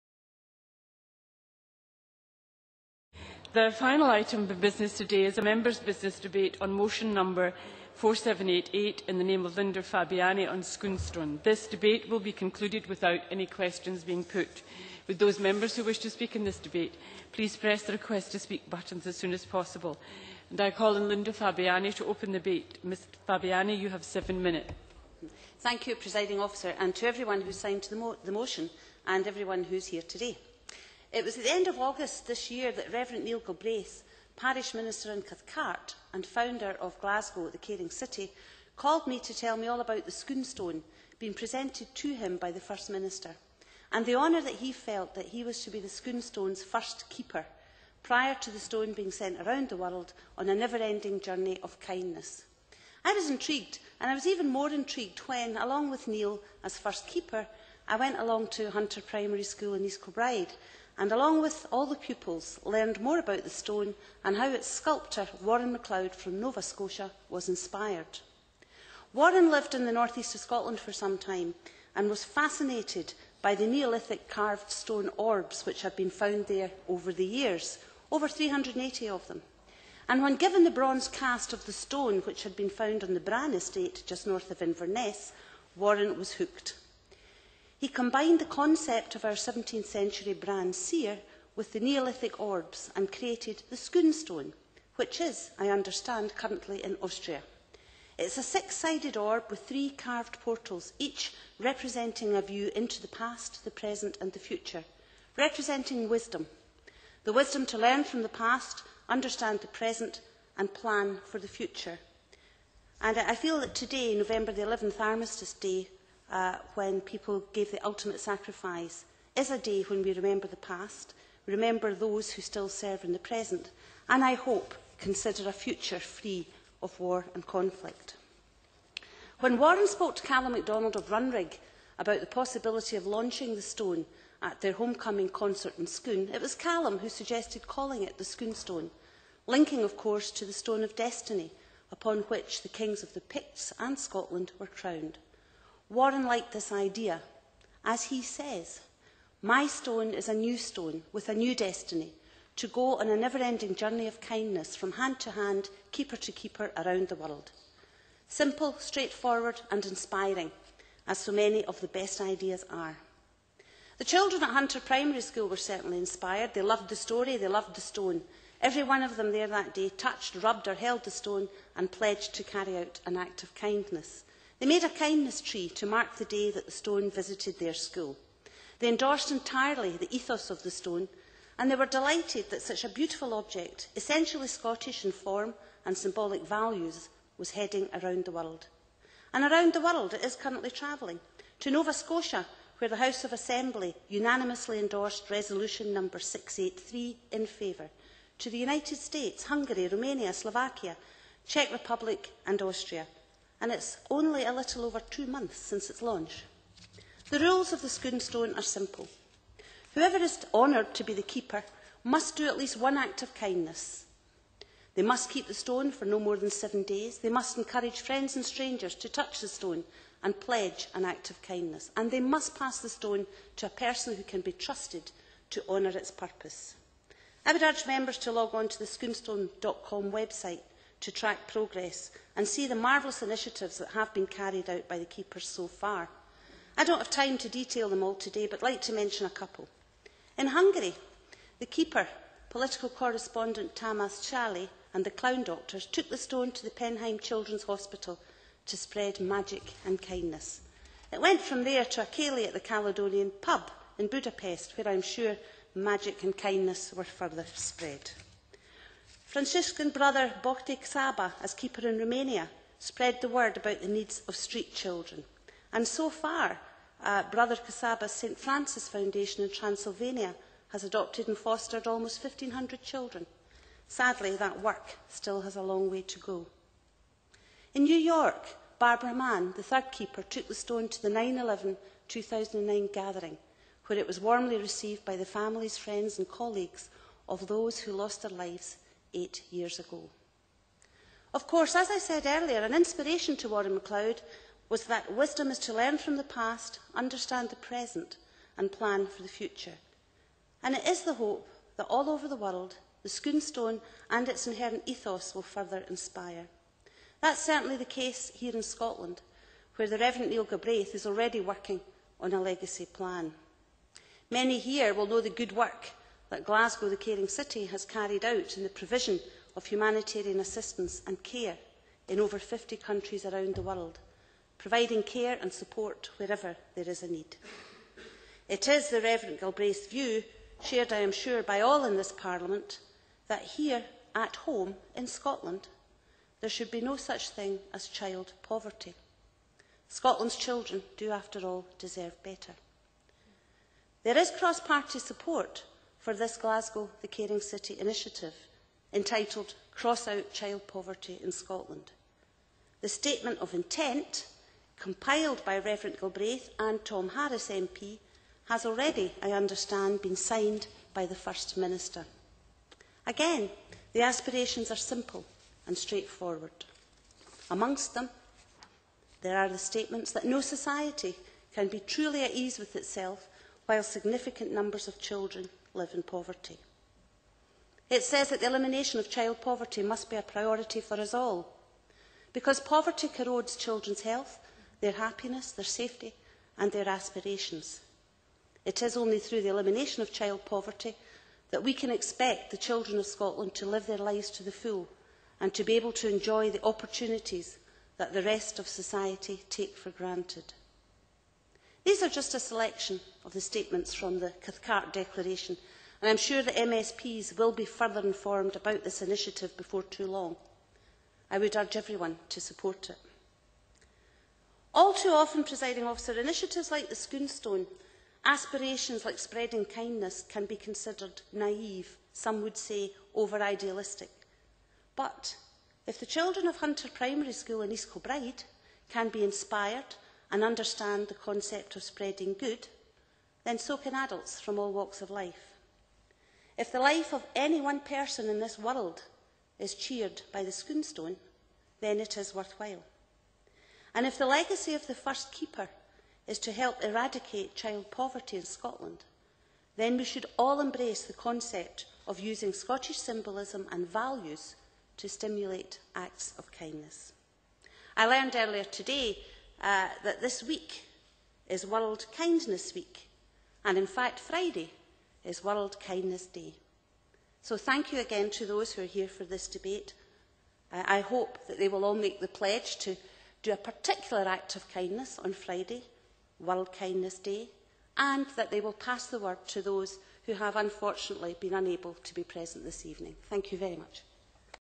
Sconestone Scottish Parliament Debate - Video 1 of 5
On November 11th, 2009, Linda Fabiani, A Scottish MSP tabled a motion in the Scottish Parliament to commend the Sconestone and its Journey of Kindness